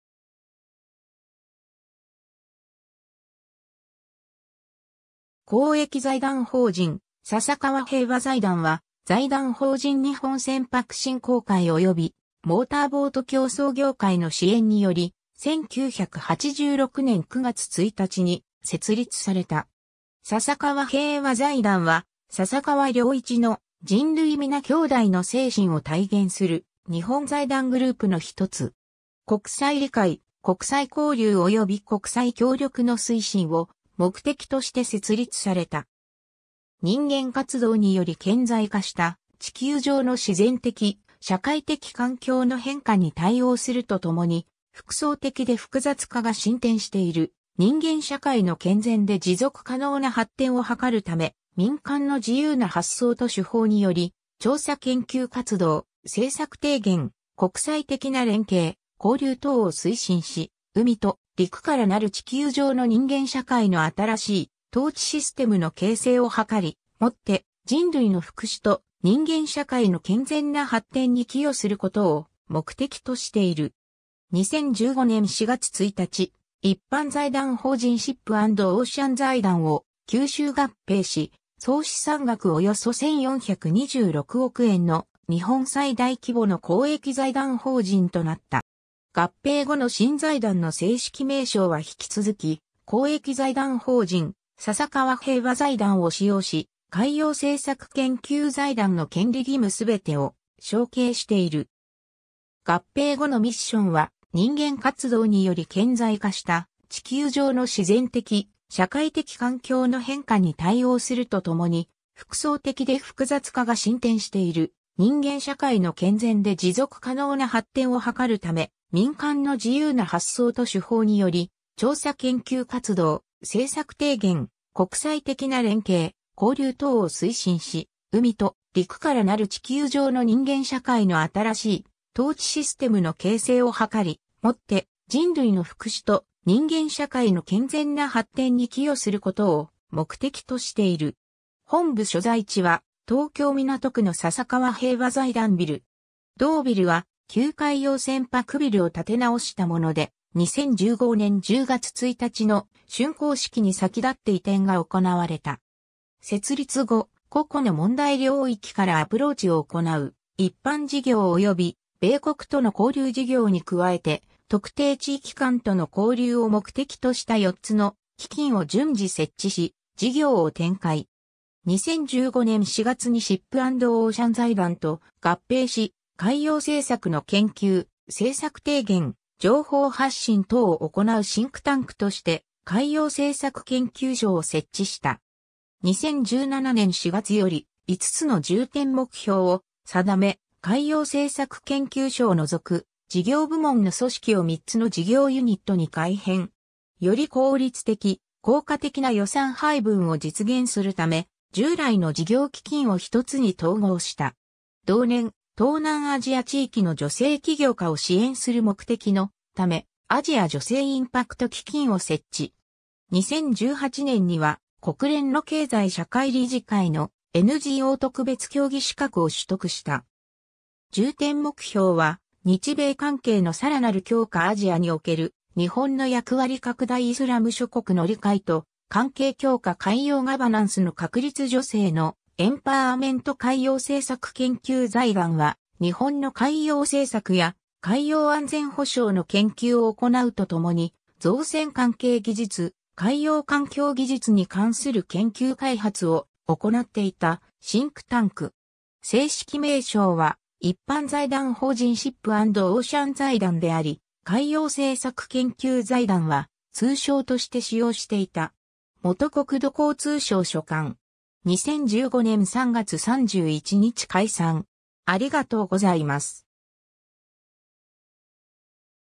笹川平和財団
公益財団法人笹川平和財団（ささかわへいわざいだん、英：The Sasakawa Peace Foundation）は、財団法人日本船舶振興会（現日本財団）及びモーターボート競走業界の支援により、1986年9月1日に設立された。
 笹川平和財団は、笹川良一の『人類みな兄弟』の精神を体現する日本財団グループのひとつ。
国際理解、国際交流及び国際協力の推進を目的として設立された。
人間活動により顕在化した地球上の自然的・社会的環境の変化に対応するとともに、複層的で複雑化が進展している人間社会の健全で持続可能な発展を図るため、民間の自由な発想と手法により、調査研究活動、政策提言、国際的な連携・交流等を推進し、海と陸からなる地球上の人間社会の新しい統治システムの形成を図り、もって人類の福祉と人間社会の健全な発展に寄与することを目的としている。
 2015年4月1日、一般財団法人シップ・アンド・オーシャン財団（通称：海洋政策研究財団）を吸収合併し、総資産額およそ1426億円の日本最大規模の公益財団法人となった。
合併後の新財団の正式名称は引き続き「公益財団法人笹川平和財団」を使用し、海洋政策研究財団の権利義務すべてを承継している。
合併後のミッションは、人間活動により顕在化した地球上の自然的・社会的環境の変化に対応するとともに、複層的で複雑化が進展している人間社会の健全で持続可能な発展を図るため、民間の自由な発想と手法により、調査研究活動、政策提言、国際的な連携・交流等を推進し、海と陸からなる地球上の人間社会の新しい統治システムの形成を図り、もって人類の福祉と人間社会の健全な発展に寄与することを目的としている。
本部所在地は東京港区の笹川平和財団ビル。
同ビルは旧海洋船舶ビルを建直したもので、2015年10月1日の竣工式に先立って移転が行われた。
 設立後、個々の問題領域からアプローチを行う一般事業及び米国との交流事業に加えて、特定地域間との交流を目的とした４つの基金（笹川太平洋島嶼国基金、笹川日中友好基金、笹川中東イスラム基金、笹川汎アジア基金）を順次設置し、事業を展開。
2015年4月にシップ・アンド・オーシャン財団（通称：海洋政策研究財団）と合併し、海洋政策の研究、政策提言、情報発信等を行うシンクタンクとして海洋政策研究所を設置した。
2017年4月より、５つの重点目標を定め、海洋政策研究所を除く事業部門の組織を3つの事業ユニットに改編。
より効率的・効果的な予算配分を実現するため、従来の事業基金を一つに統合した。
同年、東南アジア地域の女性起業家を支援する目的のためアジア女性インパクト基金を設置。
2018年には、国連の経済社会理事会（ECOSOC）のNGO特別協議資格を取得した。
重点目標は、（１）日米関係のさらなる強化（２）アジアにおける日本の役割拡大（３）イスラム諸国の理解と関係強化（４）海洋ガバナンスの確立（５）女性のエンパワーメント 海洋政策研究財団（かいようせいさくけんきゅうざいだん、英:Ocean Policy Research Foundation）は日本の海洋政策や海洋安全保障の研究を行うとともに、造船関係技術、海洋環境技術に関する研究開発を行っていたシンクタンク。
正式名称は一般財団法人シップ・アンド・オーシャン財団であり、海洋政策研究財団は通称として使用していた。
元国土交通省所管。
2015年3月31日解散。